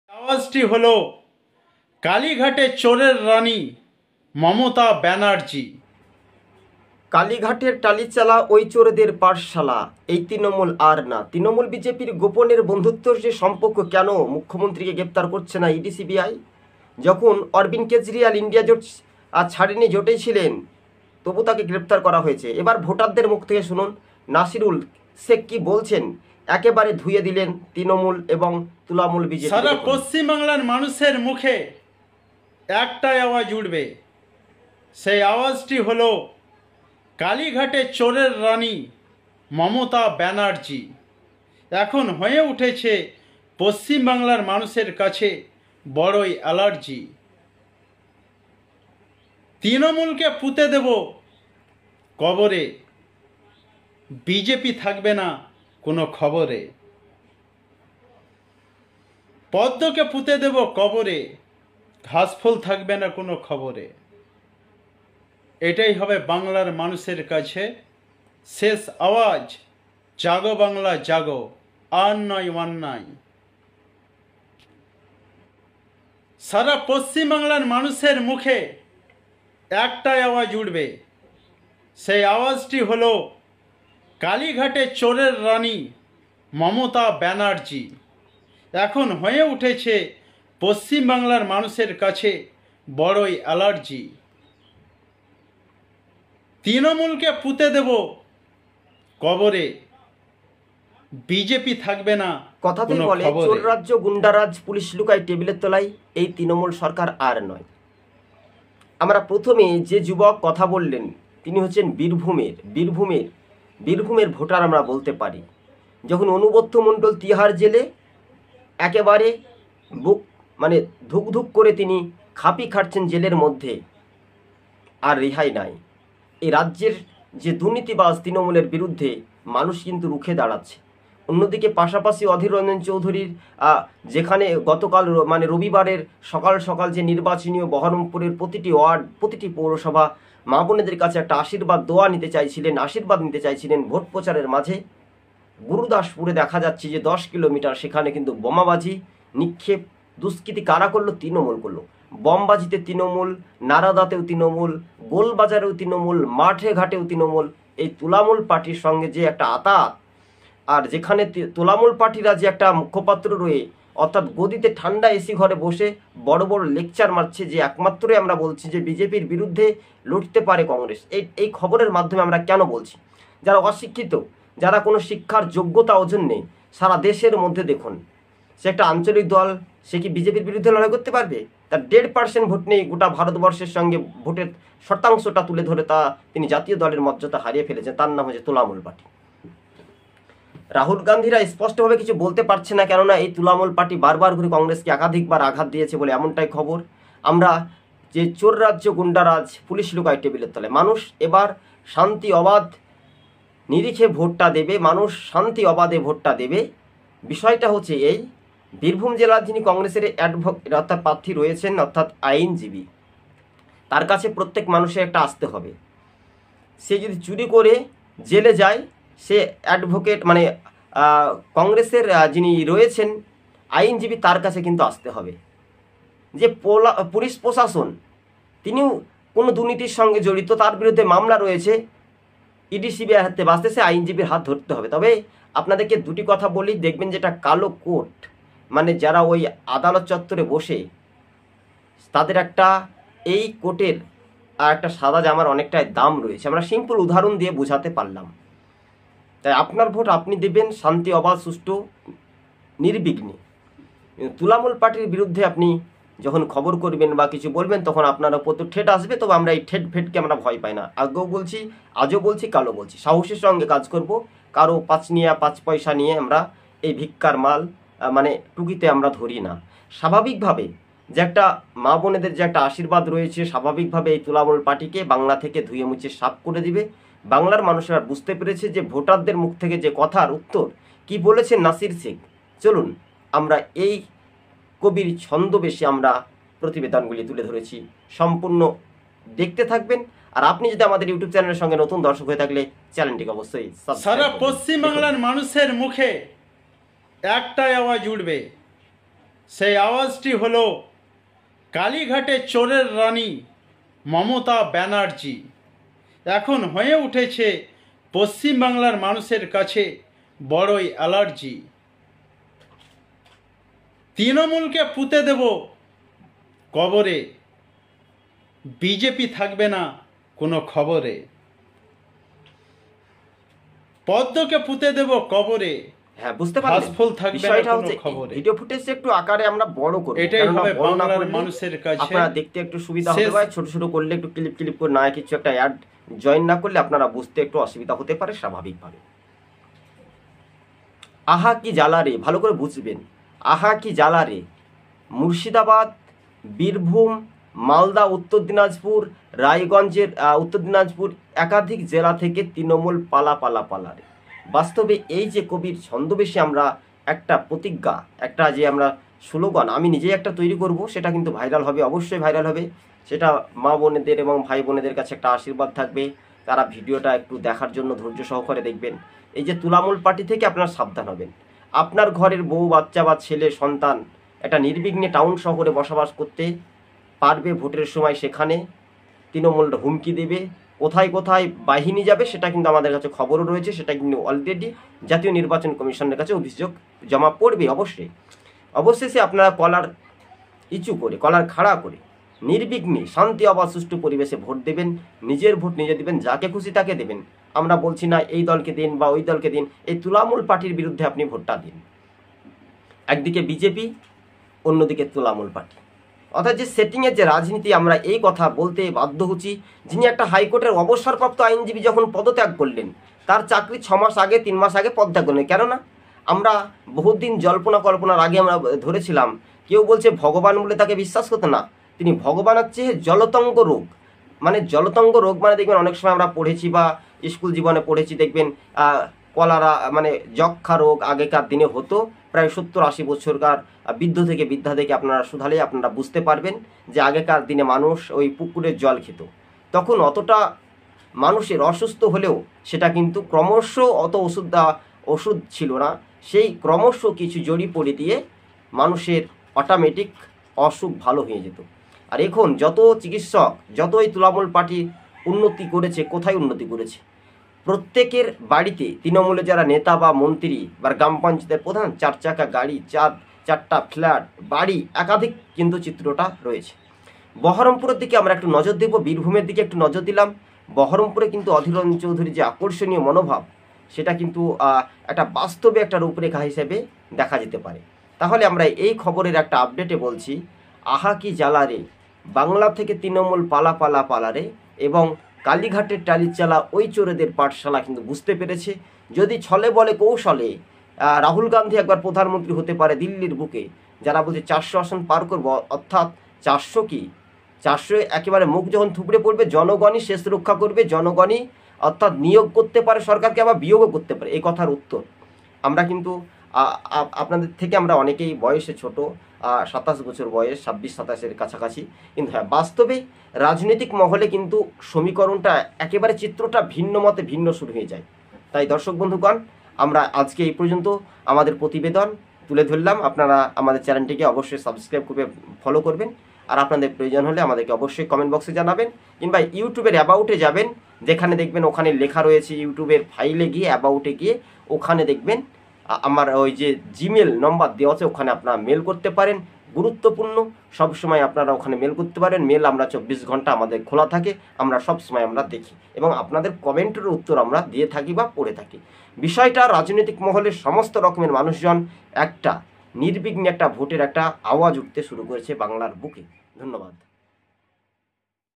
ভোটারটাই প্রতিরোধ করে নতুন স্লোগান। লোকসভা ভোটের মুখেই চিত্র দেখা গেল ভাইরাল ভিডিও।
বীরভূম মুর্শিদাবাদে জাগছে বাম কংগ্রেস জোটের।